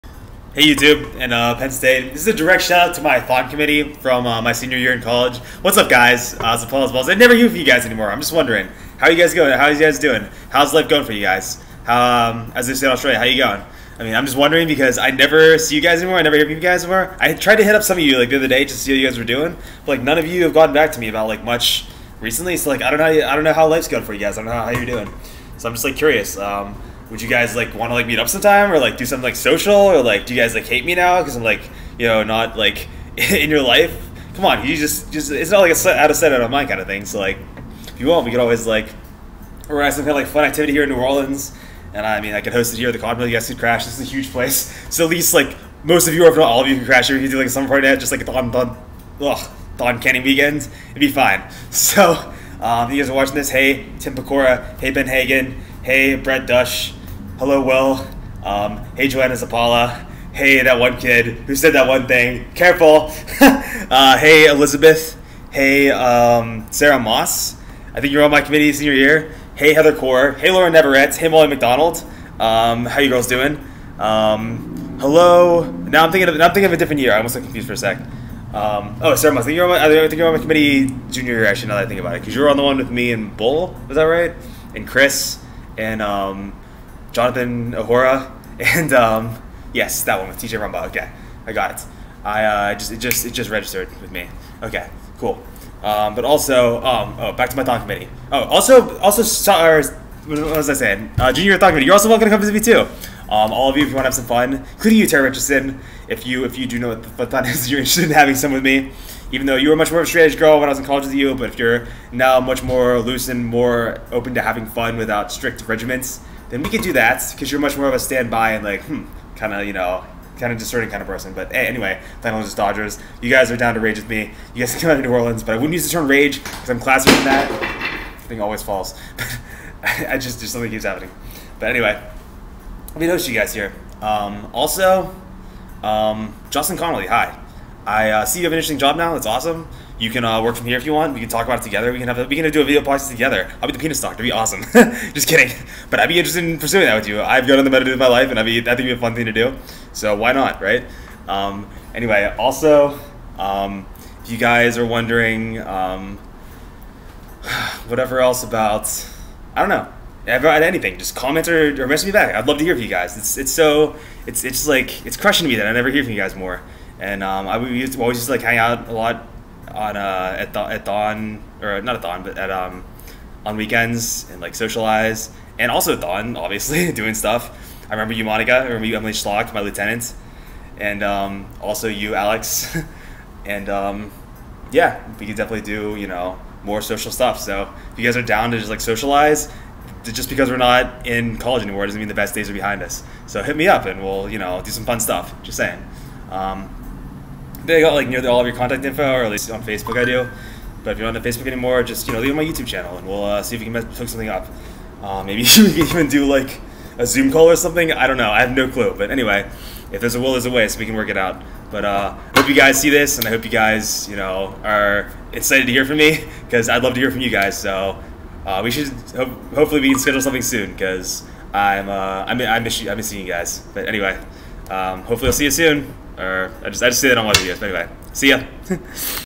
An open letter of Love and loss to my Thon Committees Zoppallaz and the THONAngeles Dodgers
Hey Guys and Gals from my OPP committe I miss all of youi and we should chill soon/ hang out.  Please get back to me?  Im dying here in New Orleans without hearing from you.